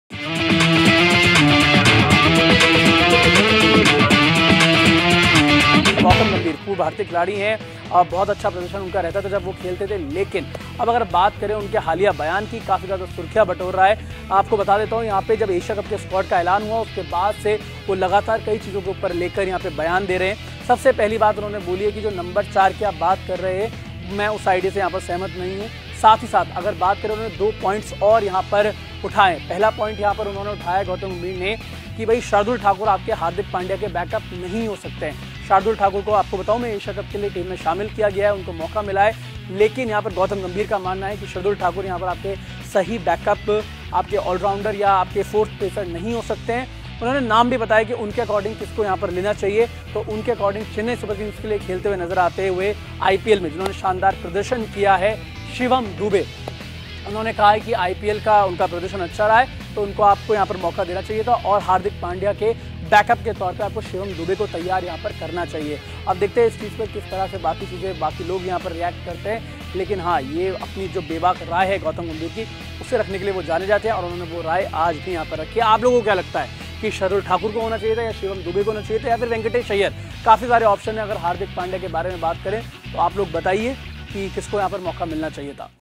Asia Cup 2023 में Shardul Thakur को मिली जगह पर पूर्व भारतीय खिलाड़ी ने दिया चौंकाने वाला बयान
पूर्व भारतीय खिलाड़ी Gautam Gambhir का कहना है की Asia Cup के लिए चुने गए Shardul Thakur Hardik Pandya के सही बैकअप नहीं उनकी जगह Shivam Dubey को मौका मिलना चाहिए था। आखिर अपने इस बयान की गंभीर ने क्या वजह बताई जानने के लिए देखें ये वीडियो।